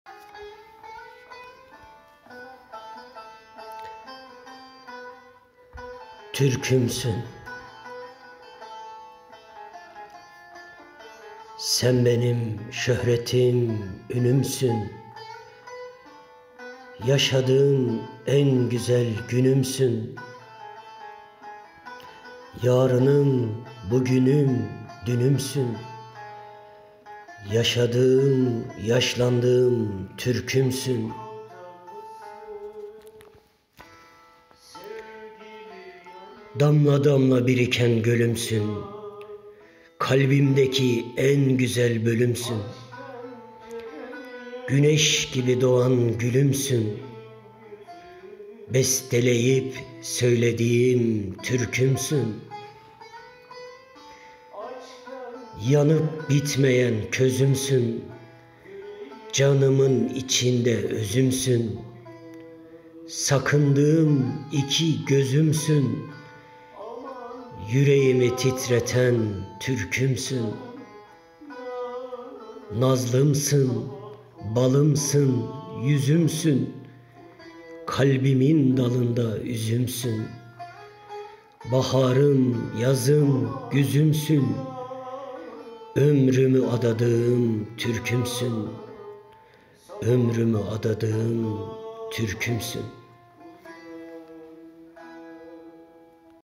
Türkümsün/Şiir:Ömer Faruk ÇAKMAK
.TÜRKÜMSÜN.
Şiir:Ömer Faruk ÇAKMAK

Sen benim şöhretim,ünümsün
Yaşadığım en güzel günümsün
Yarınım bugünüm dünümsün
Yaşadığım,yaşlandığım TÜRKÜMSÜN

Damla damla biriken gölümsün
Kalbimdeki en güzel bölümsün
Güneş gibi doğan gülümsün
Besteleyip söylediğim TÜRKÜMSÜN

Yanıp bitmeyen közümsün
Canımın içinde  özümsün
Sakındığım iki gözümsün
Yüreğimi titreten TÜRKÜMSÜN

NAZLIMSIN balımsın yüzümsün
Kalbimin dalında üzümsün
Baharım yazım güzümsün
Ömrümü adadığım TÜRKÜMSÜN